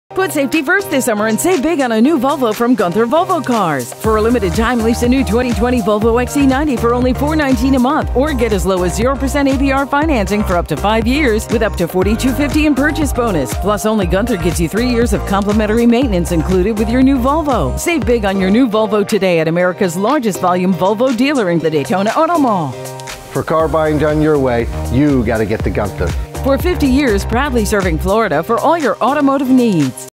Gunther Volvo Cars Daytona Beach XC90 Commercial August 2020
Put safety first this Summer
And save big on a new Volvo from Gunther Volvo Cars.
For a limited time,
Lease a new 2020 Volvo XC90 for only $419 a month
Or get as low as 0% APR financing for up to 5 years.
With up to $4,250 in Purchase bonus.
Plus only Gunther gives you 3 years of complimentary maintenance included with your new Volvo.
4)Save big on your new Volvo today at America’s Largest Volume Volvo Dealer
In the Daytona Auto Mall.
For car buying done your way,
You gotta get to Gunther.
For 50 years proudly serving Florida for all your automotive needs.